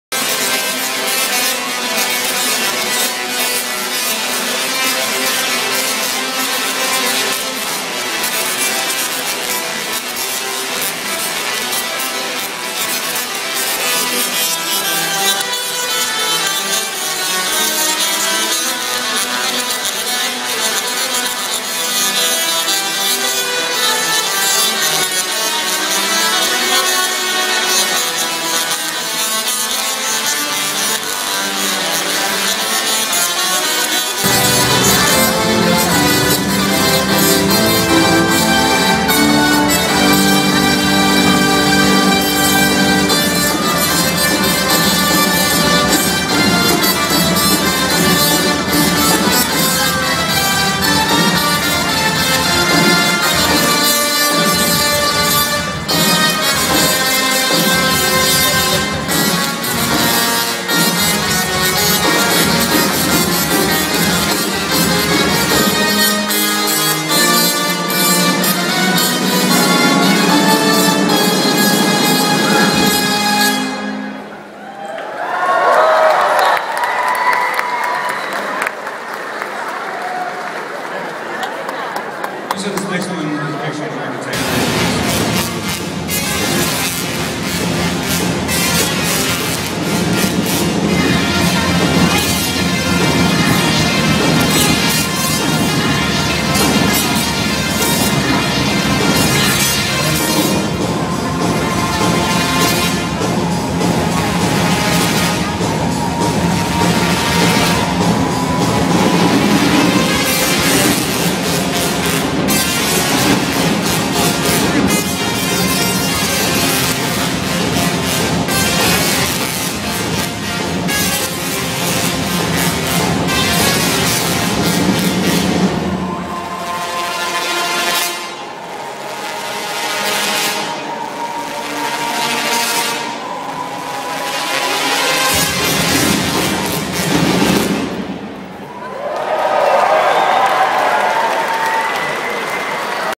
Tesla's song
Two Tesla coils playing music at the 2011 Maker Faire, San Mateo, CA